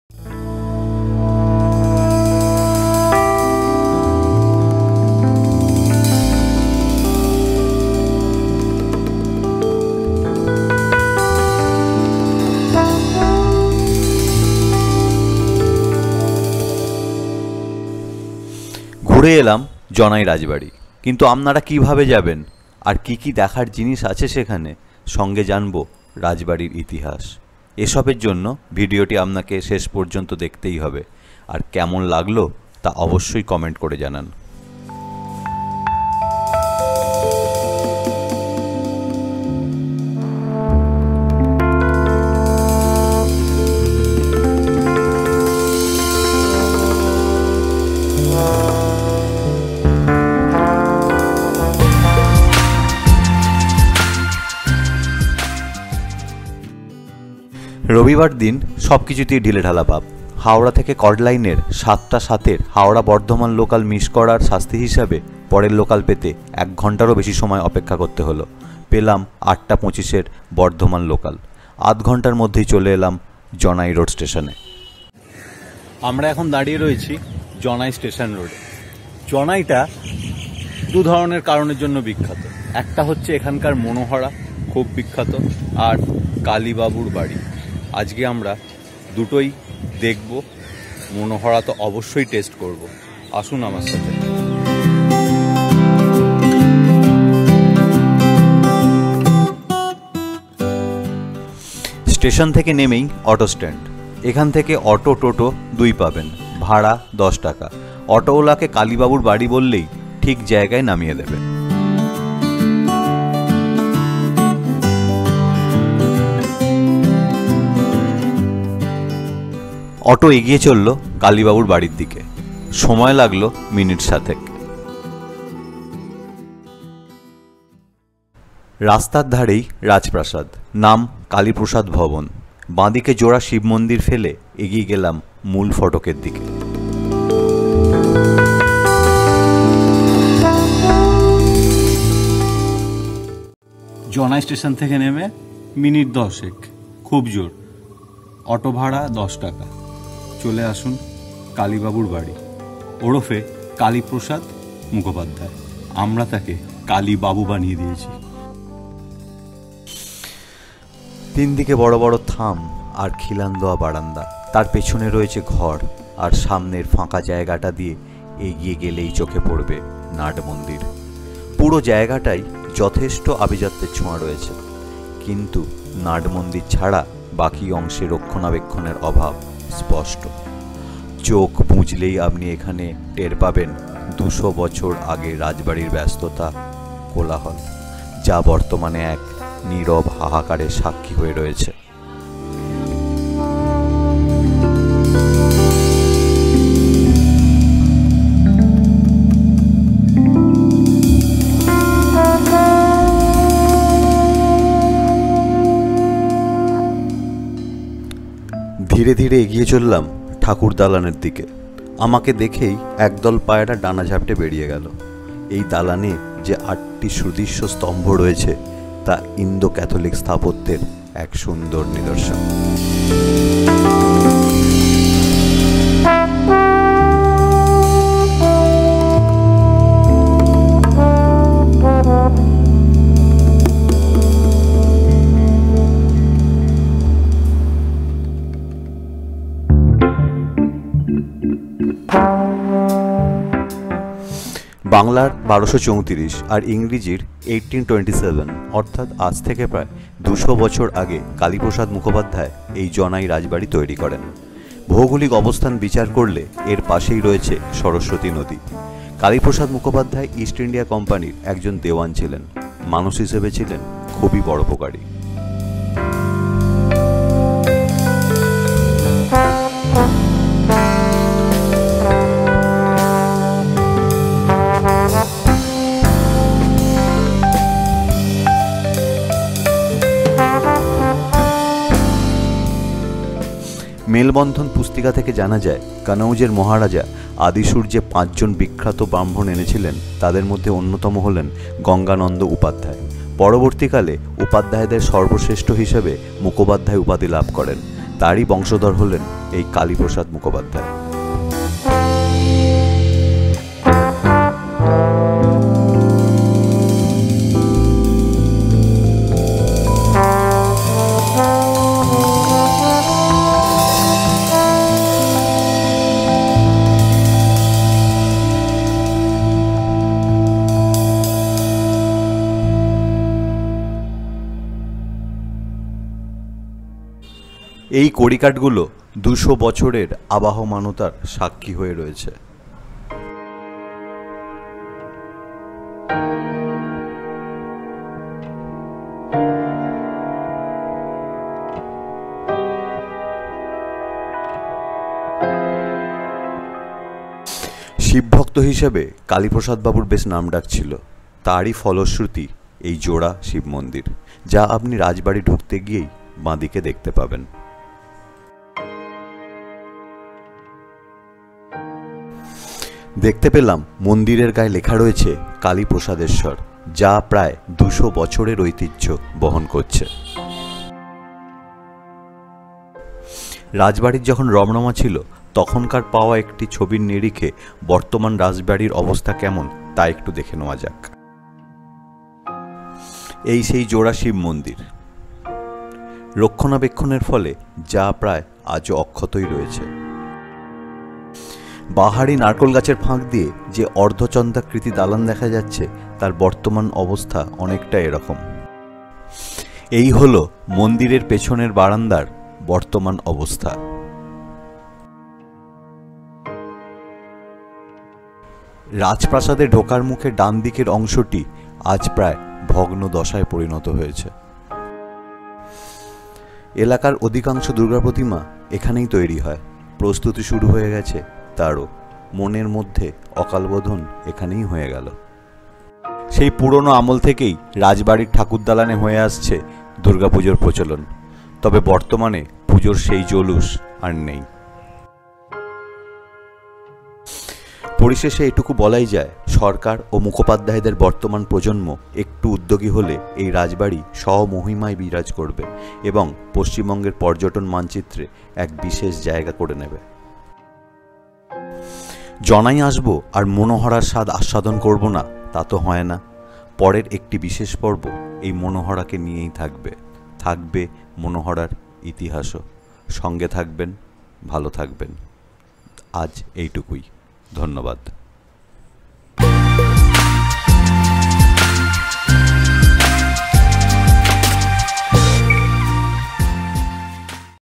Janai Rajbari | Kali babur bari| জানাই রাজবাড়ী |Hoogly
According to the information available in the "Malebandhan" Pustika, Maharaja Adisur From Kanauj brought with him five most renowned Brahmins among which the most famous was Gangananda who was a disciple of Shri Harsha and it is during this period that Debibar Ghatak created "Malebandhan" and added Upadhyay as the surname, which later modified into Mukhopadhyay (supreme of all the upadhyay's) or Mukherjee(in the later British era).As such Gangananda Bhattacharya Upadhayay was the 1st man in the famous family of Janai.

The two reasons for the popularity of Janai village includes: 1. A sweet named "Monohara" that is famous in all over Bengal, and 2; the story of assessts, grandeur and kindness of the Royal Family of Janai.